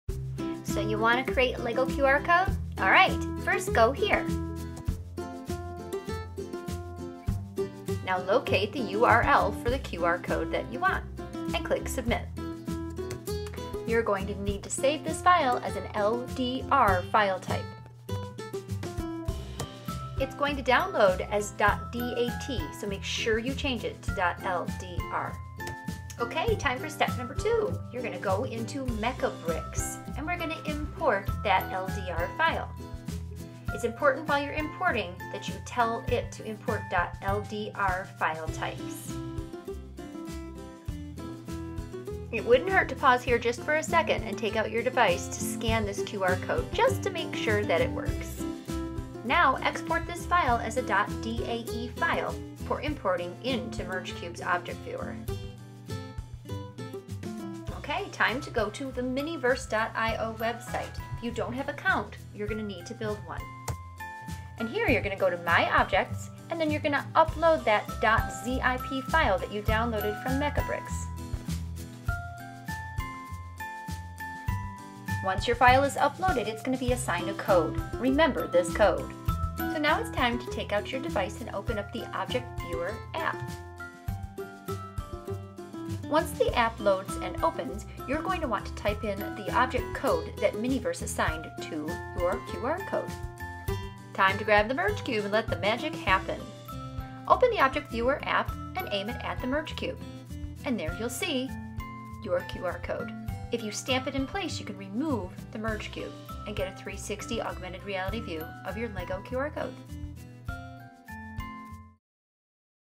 QR to Lego to Object Viewer on Merge Cube
This video provides step-by-step directions how to create a LEGO QR Code 3D Model AND how to use Object Viewer for Merge Cube.

Don't have a Merge Cube?

What is Merge Cube?

Need Merge Cube Lessons for your classroom?